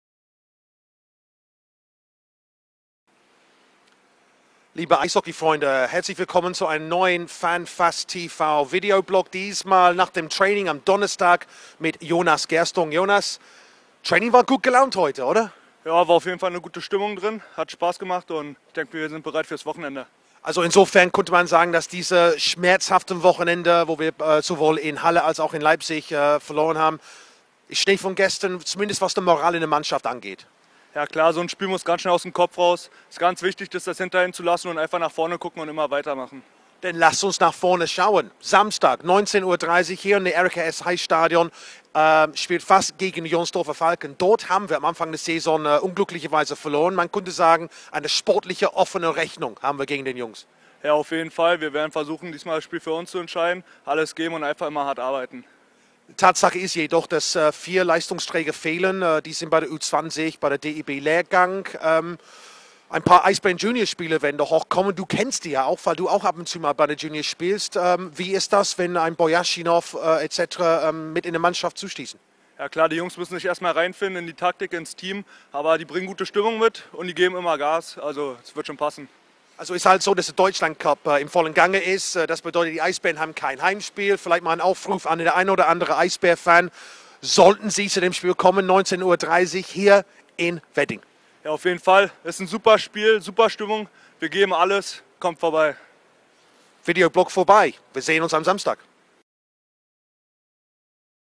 FassFanTv - Interview mit Jonas Gerstung
Des Squire im Gespräch mit Verteidiger Jonas Gerstung über das Heimspiel F.A.S.S. Berlin vs. EHC Jonsdorfer Falken am Wochenende (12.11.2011).